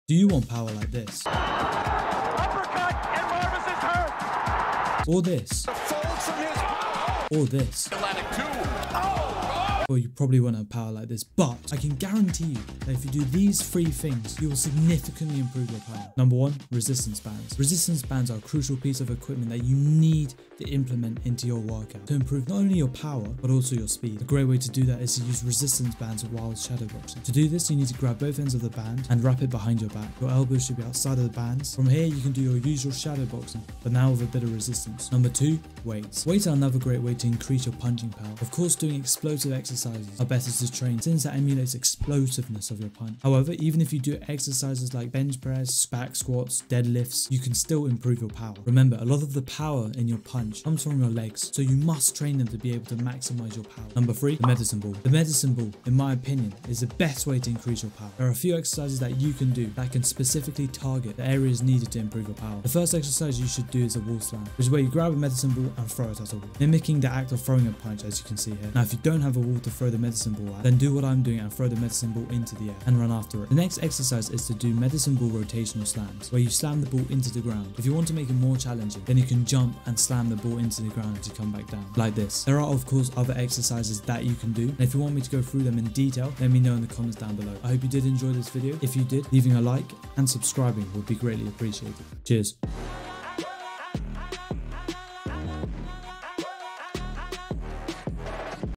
The BEST EXERCISES to 10X Your POWER In Boxing 🥊
In this video I Show YOU The 3 BEST WAYS to INCREASE your POWER.these methods are Extremely useful and I use them myself to improve my power.